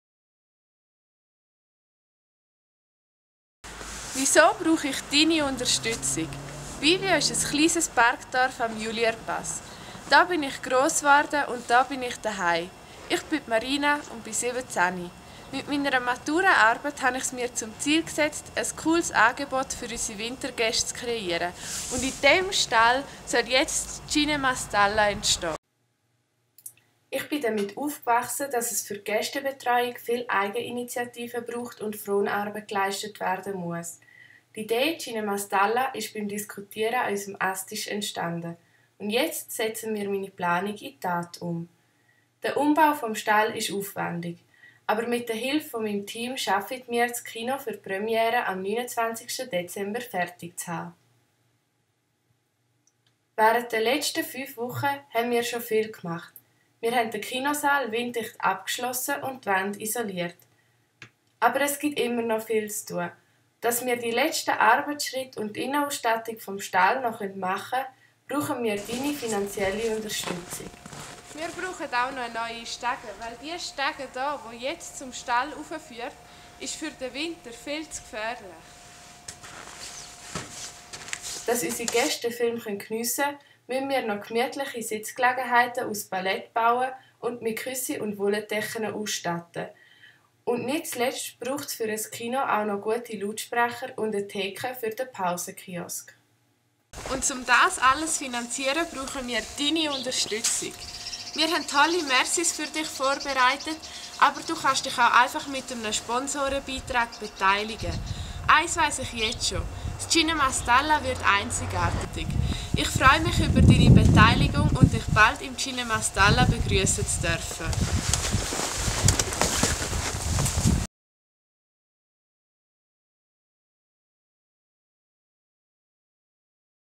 CinemaStalla
Im kleinen Bündner Bergdorf Bivio entsteht ein Kino der besonderen Art.
Ein ausgedienter Heustall wird in Fronarbeit zum Kino umgebaut. In heimeliger Atmosphäre zeigen wir Filme wie «Schellenursli», «Heidi» oder «Unerhört Jenisch», die perfekt in die Berge und zum Kanton Graubünden passen. Eine persönliche Begrüssung, ein eigener Kurzfilm zum Projekt und ein Pausenkiosk mit persönlicher Note vervollständigen das Kinoerlebnis.